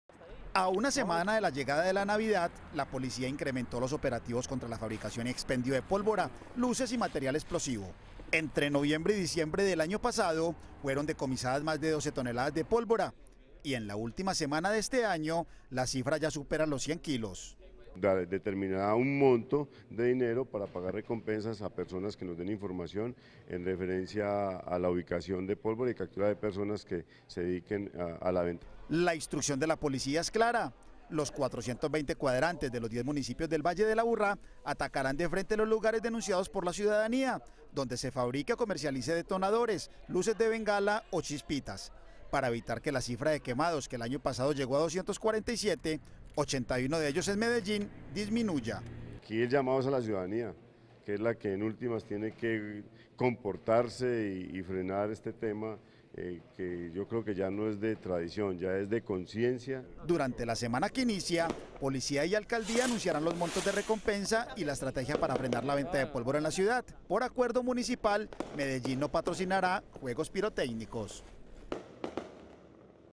La Policía anunció que ha decomisado más de 100 kilos de pólvora [Noticias] - TeleMedellin
A una semana de la llegada de navidad, la Policía incrementó los operativos contra la fabricación y expendio de pólvora, luces y material explosivo. En los últimos tres meses de 2013, fueron decomisadas más de doce toneladas de pólvora y en la última semana de este año, la cifra ya supera los 100 kilos.